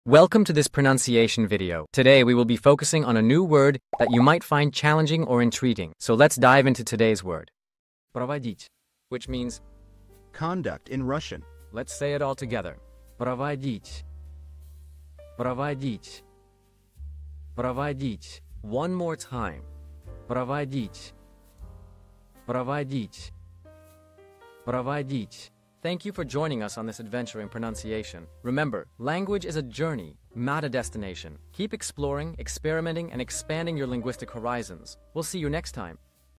How to pronounce Provodit' | Проводить (Conduct in Russian)
Master the Pronunciation of 'Provodit' | Проводить' - which means : Conduct in Russian 📝 with @PronunciationRussian 🎓🗣️ - [Your Guide to Russian]

Learn to pronounce 'Provodit'' perfectly in Russian with our easy and engaging tutorial by @PronunciationRussian! 📖📽️ In this comprehensive video, we provide you with native speaker examples 🗣️👂 and phonetic breakdowns 🎵 to ensure you get the pronunciation just right.